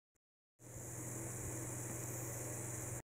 Motharina Screamed out Serhad's Name
Motharina Doesn't want revenge on Serhad. hse is still friends with him. its just like maul screaming out Kenobi. this is the most important thing